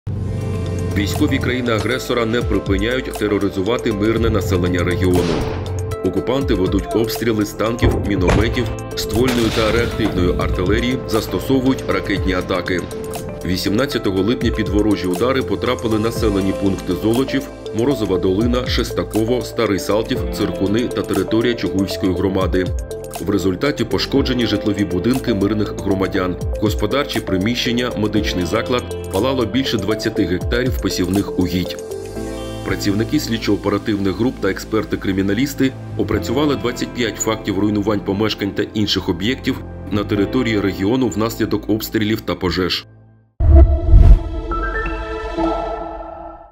Харків 19 липня. Оперативно від поліції на ранок по місту та області
Військові країни-агресора не припиняють тероризувати мирне населення регіону. Окупанти ведуть обстріли з танків, мінометів, ствольної та реактивної артилерії, застосовують ракетні атаки.
18 липня під ворожі удари потрапили населені пункти Золочів, Морозова Долина, Шестаково, Старий Салтів, Циркуни та територія Чугуївської громади. В результаті пошкоджені житлові будинки мирних громадян, господарчі приміщення, медичний заклад, палало більше 20 га посівних угідь. Працівники слідчо-оперативних груп та експерти-криміналісти опрацювали 25 фактів руйнувань помешкань та інших об’єктів на території регіону внаслідок обстрілів та пожеж.